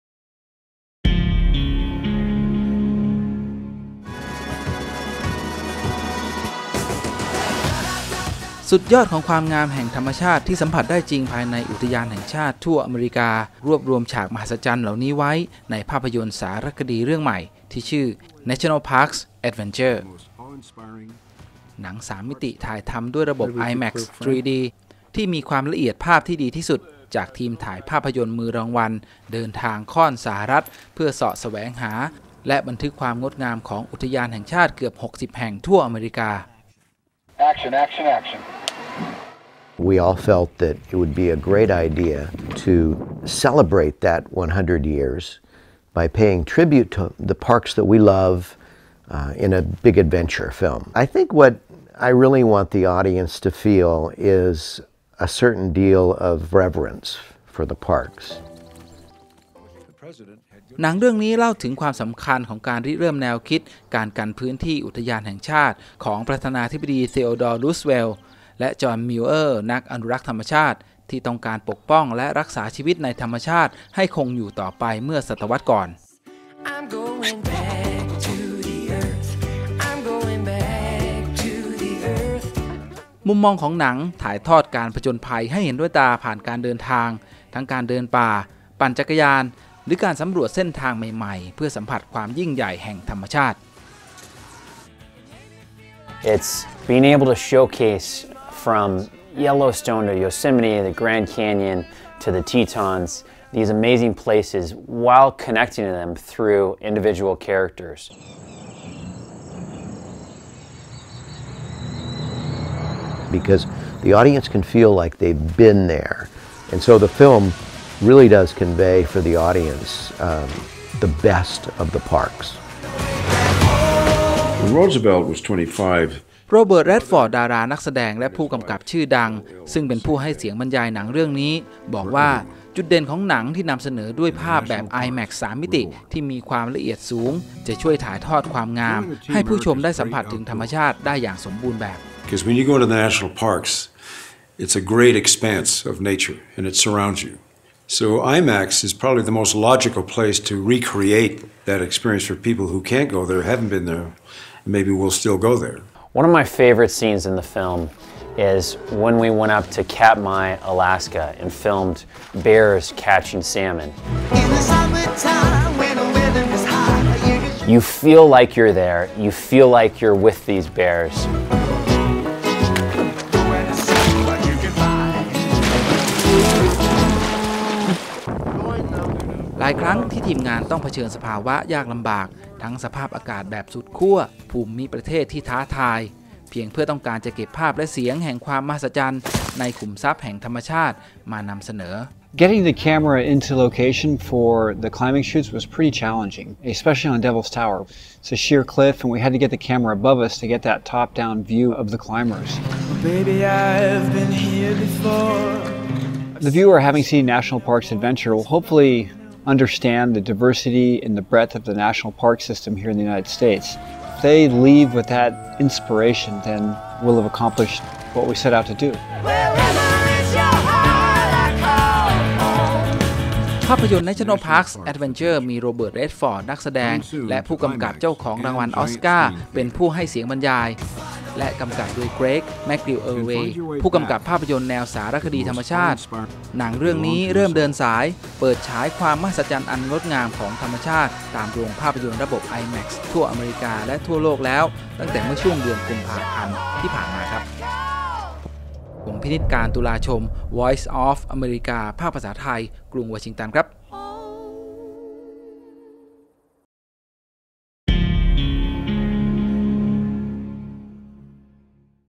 มหัศจรรย์ธรรมชาติในภาพยนตร์ฉลอง 100 ปีอุทยานแห่งชาติแห่งอเมริกา
วีโอเอ พาไปชมความงามและความมหัศจรรย์ของธรรมชาติผ่านภาพยนตร์สารคดีเรื่องใหม่ในโอกาสร่วมฉลองครบรอบ 100 ปีการประกาศเขตอุทยานแห่งชาติในสหรัฐเป็นครั้งแรกซึ่งมีส่วนช่วยพลิกฟื้นมรดกทางธรรมชาติที่เคยเสี่ยงต่อการล่มสลายให้กลับมางดงามและสมบูรณ์ในปัจจุบัน